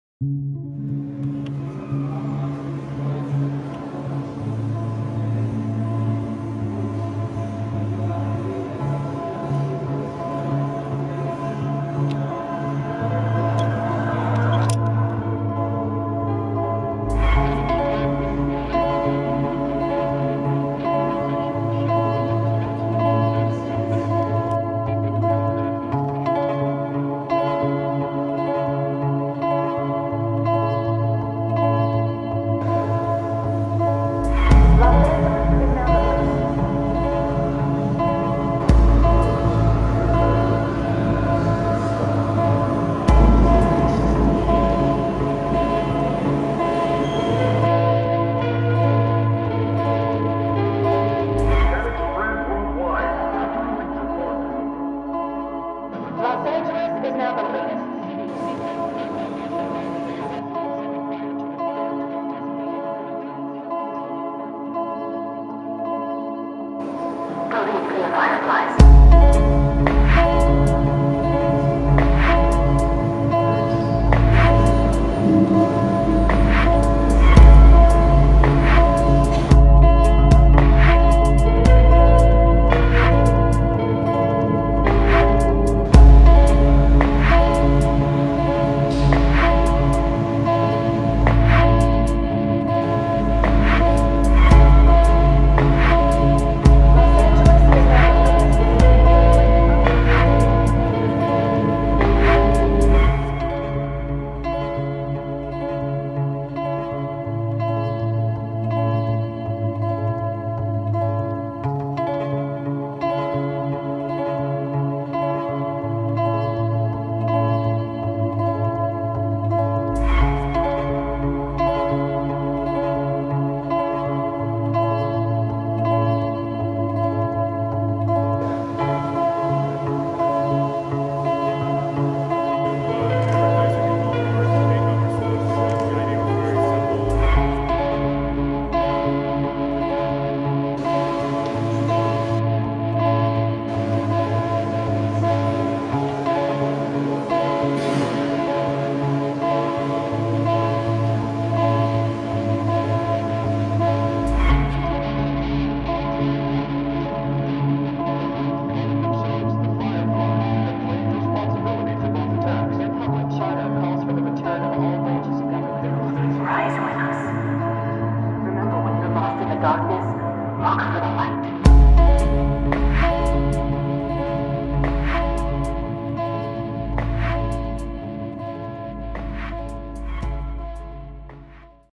Magic City - Street Art Opening in Dresden, Germany - Ben Heine Art
Magic City Street Art Collective Show Opening in Dresden, Germany.

You can view Ben's work at 2:00